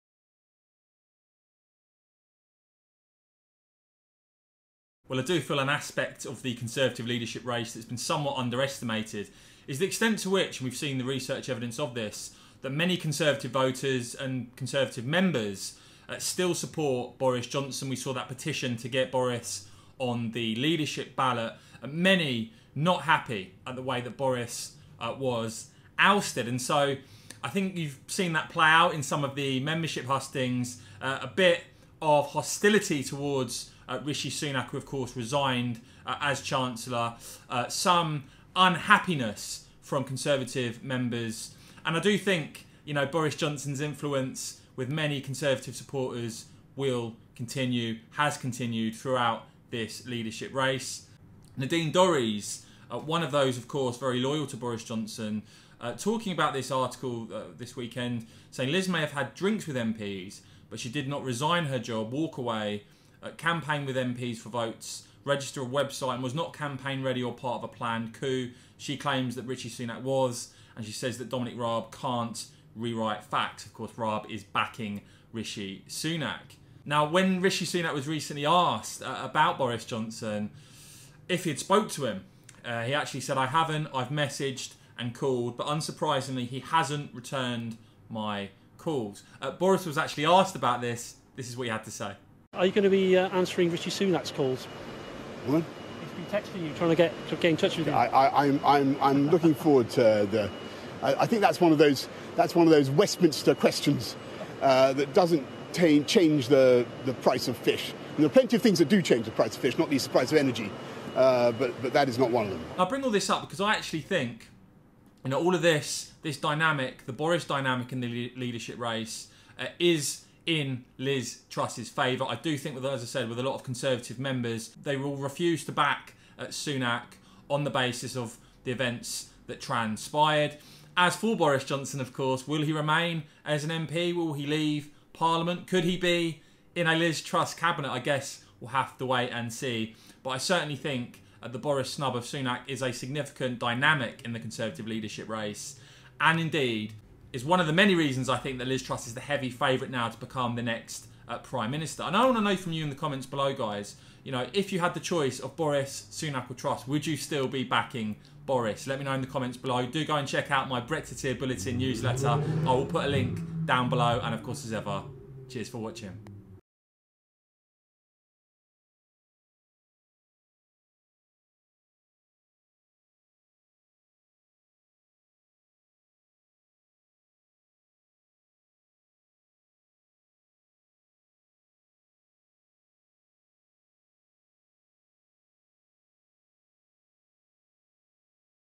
Boris Johnson SNUBS Leadership Contender
"Rishi Sunak: Boris Johnson hasn’t returned my calls since I quit Cabinet" Telegraph